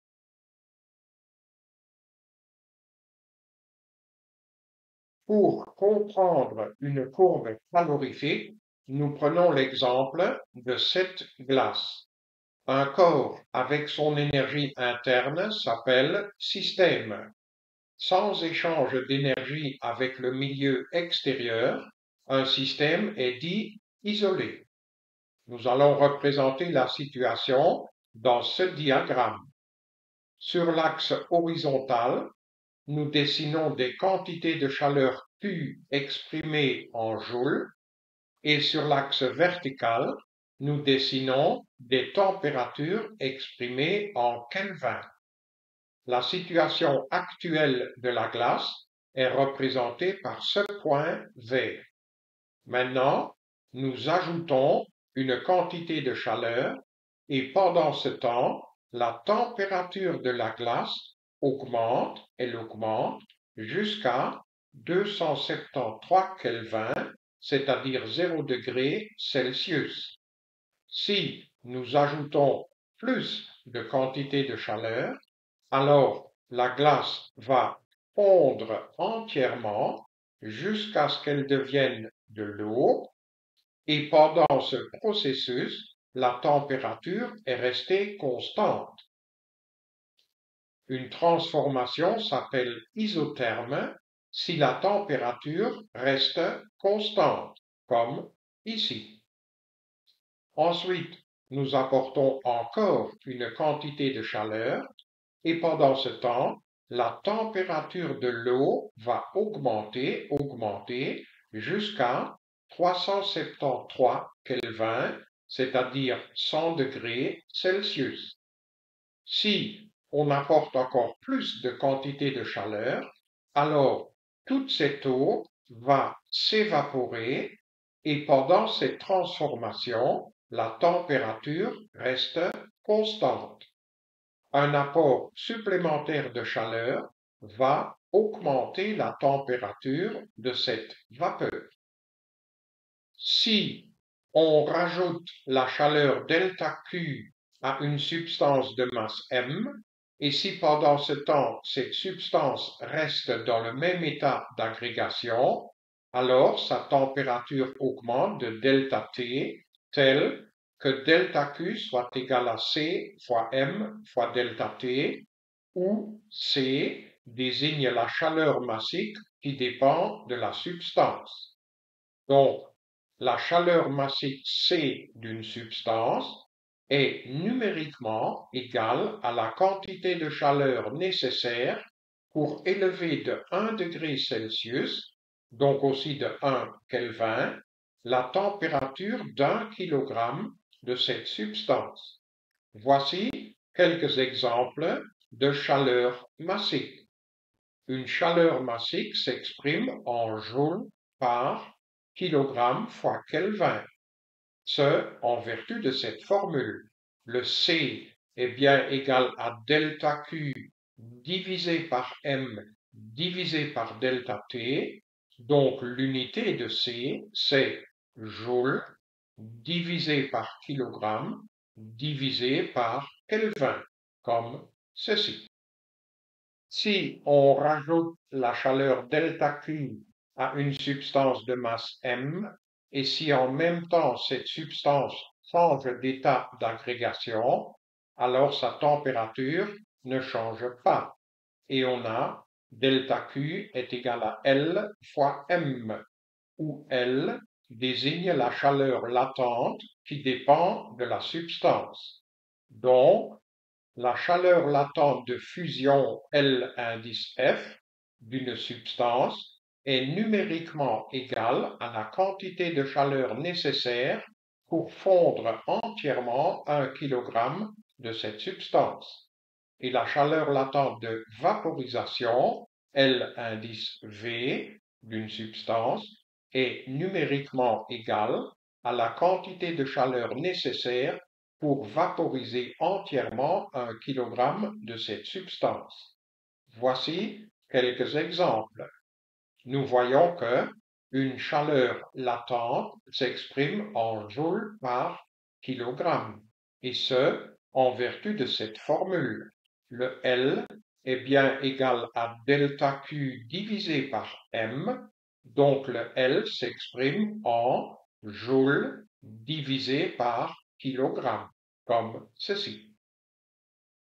Physique 6PhF005 Quantité de chaleur
Calorimétrie: comment varie la température d'une substance si on lui rajoute de la quantité de chaleur? Qu'est-ce une courbe calorifique? Qu'est-ce la chaleur massique d'une substance? Qu'est-ce la chaleur latente d'une substance?